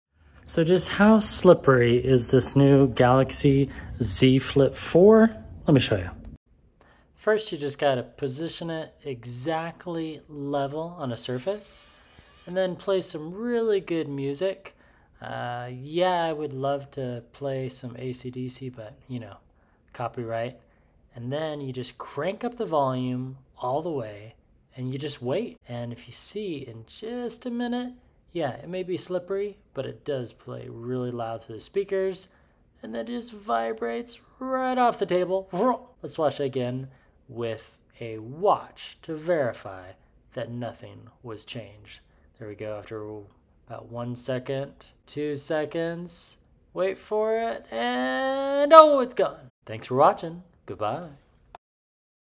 Slippery Test: Galaxy Z Flip4 by... SAMSUNG
How slippery is the Galaxy Z Flip4 smartphone by Samsung? Is a little AC DC music loud enough to slide it off a table...?

Video title lifecycle:
1. Galaxy Z Flip4 - slippery test...
2. Slide Test: Galaxy Z Flip4 by... SAMSUNG